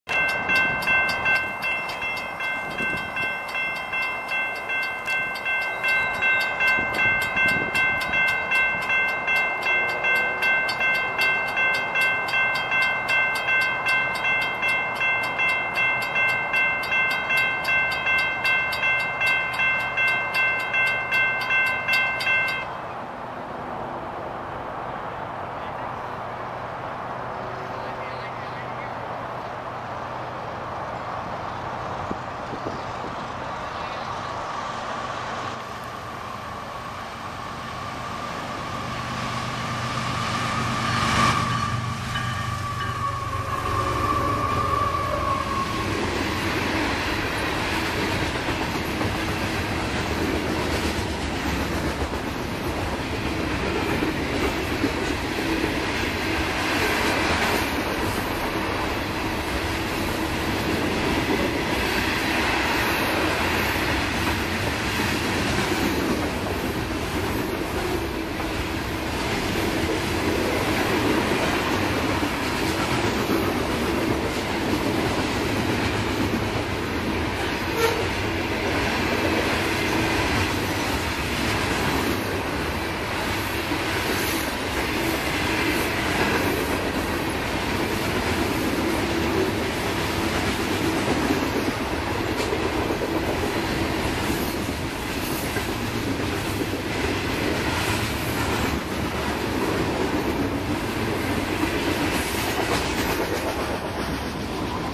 CSX 087
SB CSX K929-?? thru CP Coconut in Boynton Beach.

Power:
CSXT 870
CSXT 530

Taken 11/26/16 at 2:31 PM.